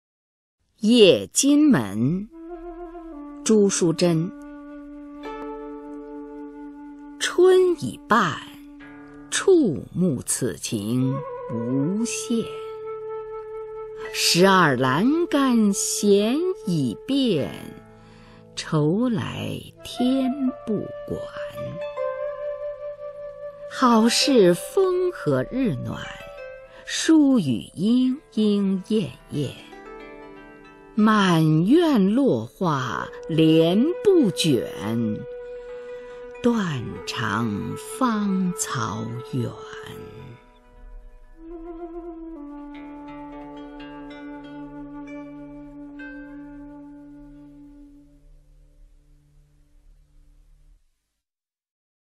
谒金门 春半 朱淑真 宋词选79 国学经典诵读
《谒金门·春半》是南宋诗人朱淑真创作的一首写闺中春愁的小词。上片写仲春时节，眼前景色，触目生愁。虽“十二栏干倚遍”，也无法排遣春愁。下片写闺中人在这风和日暖的大好春光中，想起了自己所怀念的人，不禁愁绪万端，感到还不如成双成对的鸟儿，因此不愿再看见满院落花和断肠芳草。通篇哀婉细腻，愁思无限。

白话译文
春光已匆匆过去了一半，目光所及，繁花凋落，春天将要逝去。整日斜倚栏杆，徘徊眺望，伤春逝去的愁怨，袭上心头，天也无法帮助摆脱。
风和日暖，在这么好的春光，独自倚靠斜栏旁，还不如那双双对对的莺燕。院里落满了残花，垂下幕帘呆在屋里，不忍看到春天逝去的景象。芳草漫漫到天边，思恋的人远在天边处，令人悲肠欲断。